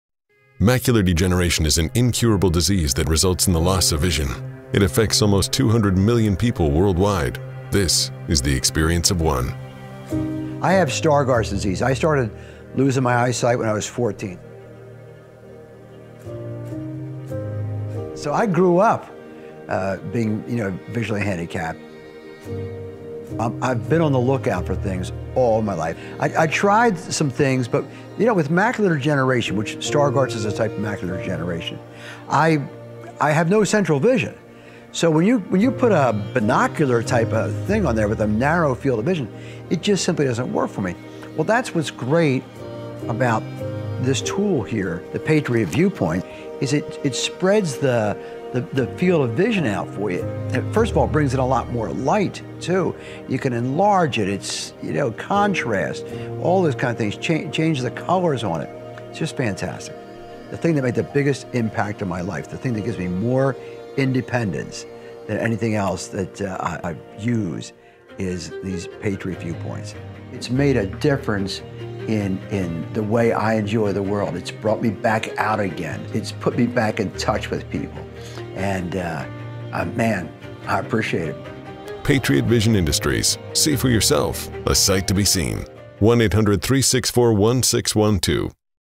How the Patriot ViewPoint Has Made a Difference in Bill Quain's Life
Bill Quain is an author, professional speaker, professor and businessman who lost his vision due to Stargardt disease at age 14. The Patriot ViewPoint has made a difference in his life by helping him to see things he hasn’t been able to in years.

Patriot Vision Industries... see for YOURSELF a sight to be seen